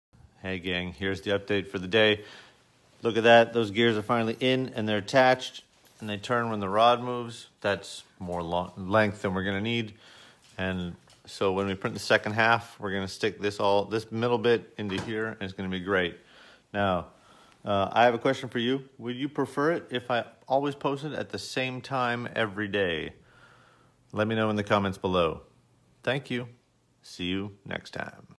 Sixi Robot : Tuning fork improvements
I call the big Y shaped part the tuning fork.  We split the bottom center into four parts so that we could reprint (correct) each piece without reprinting the bits that are fine.